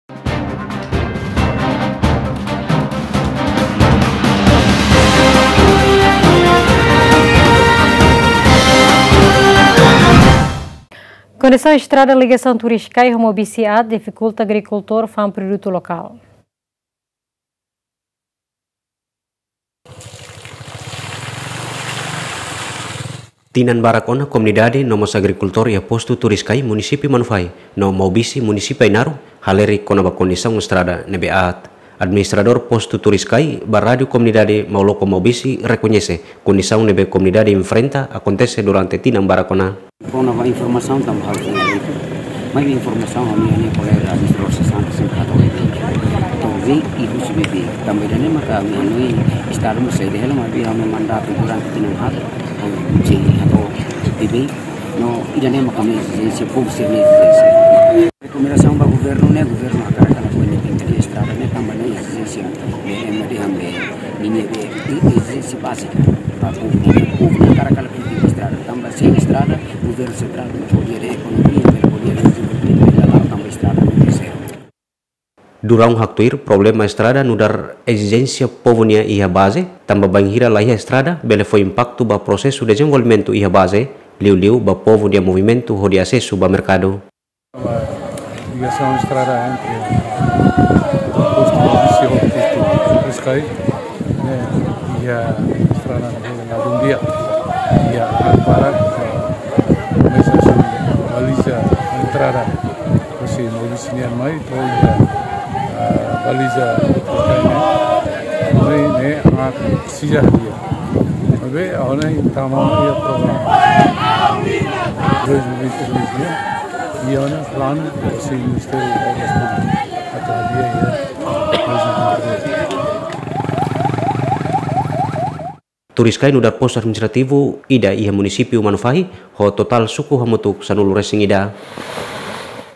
Estrada Turiscai aat, difikulta agrikultór lori produtu lokál ba merkadu
Estrada Turiscai aat, difikulta agrikultór sira lori produtu lokál ba merkadu